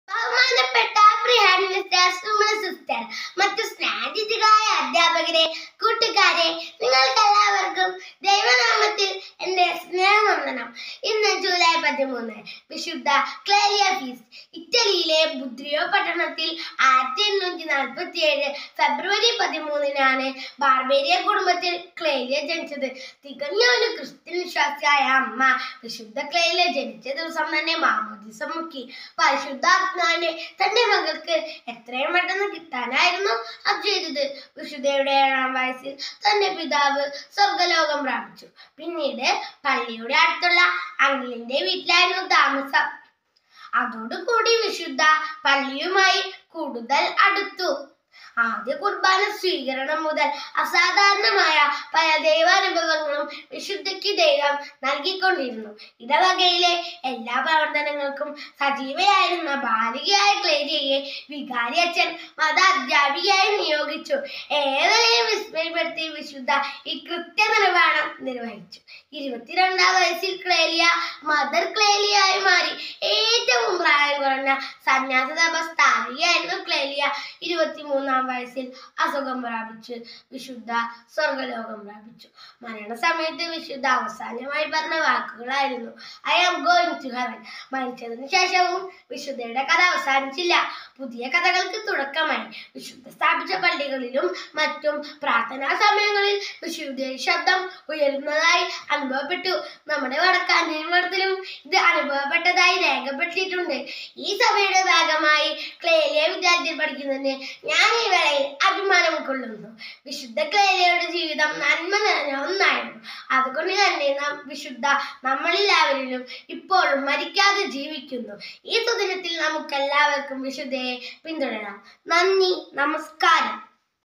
BEST PERFORMANCE IN SPEECH COMPETITION# STD 3# HAYA AYISHA#CLELIA BARBIERI HOLY ANGELS E.M.HS
BEST PERFORMANCE IN SPEECH COMPETITION IN ACCORDANCE WITH CLELIA FEAST DAY
STD 3 # HAYA AYISHA
CLELIA BARBIERI HOLY ANGELS E.M.HS WADAKKANCHERY.